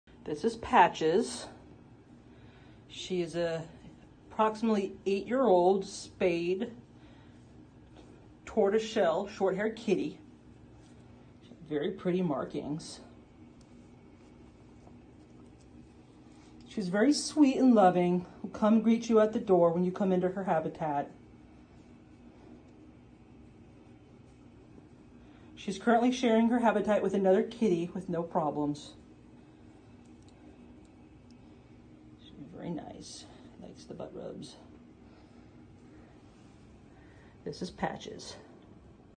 Patches A784502 (ADOPTED 8/2/20)
Sweet and loving kitty who will purr with appreciation when you pet her. She is currently sharing a habitat room with another cat without issues.

If you are interested in adopting this kitty, please call the adoption by appointment phone number at (916) 875-CATS (2287) between 9:30 a.m. to 3:30 p.m. Tuesday through Sundays (no holidays) to set up an appointment to meet this kitty. Be prepared to give the shelter representative Patches's animal ID number, which is A784502.